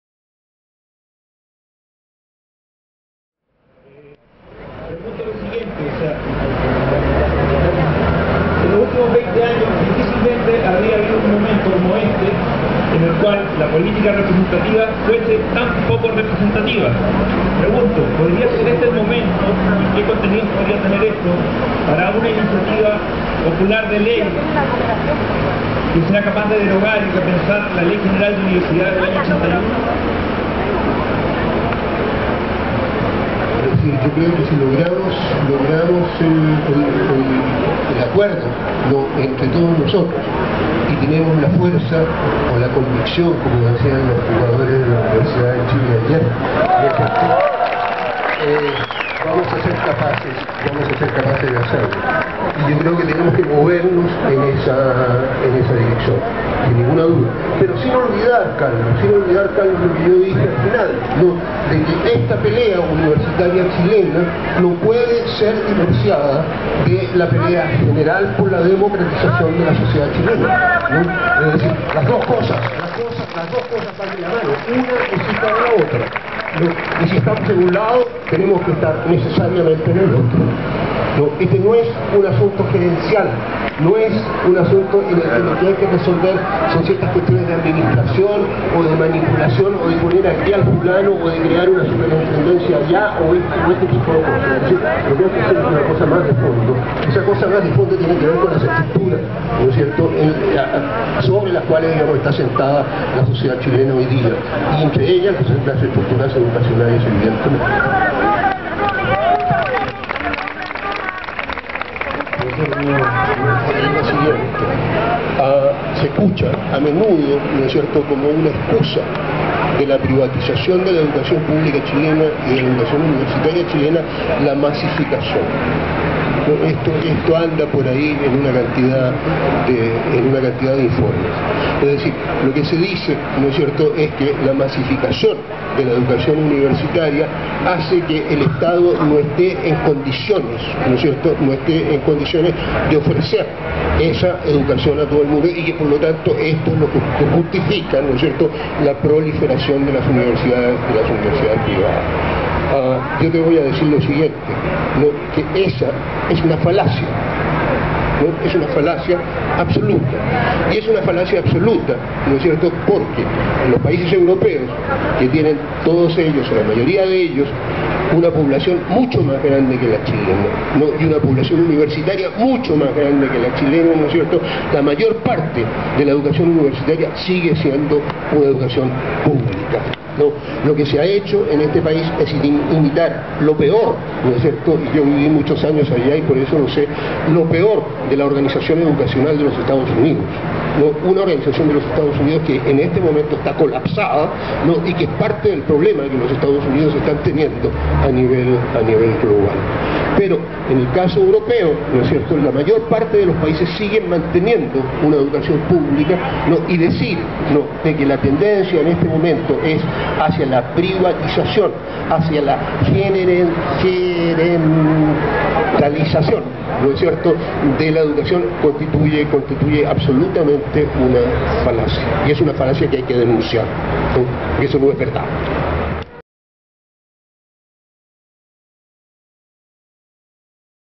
EL ESTADO NOS TIENE EN LA CALLE
Esta pelea universitaria chilena no puede ser divorciada de la pelea general por la democratización de la sociedad chilena...
Las dos cosas van de la mano.
Una es igual a la otra; Y si estamos en un lado, tenemos que estar necesariamente en el otro.
Este no es un asunto gerencial... No es  un asunto en que lo hay que resolver son ciertas cuestiones de administración, o de manipulación; o de poner aquí al fulano, o de crear una superintendencia allá...
Aquí hay que ver una cosa más de fondo, y esa cosa más de fondo son las  estructuras sobre las que debería estar sentada la sociedad chilena, y entre ellas las estructuras educacionales.
Grínor Rojo
Conversación en Plaza de Armas de Santiago de Chile
13 de junio 2011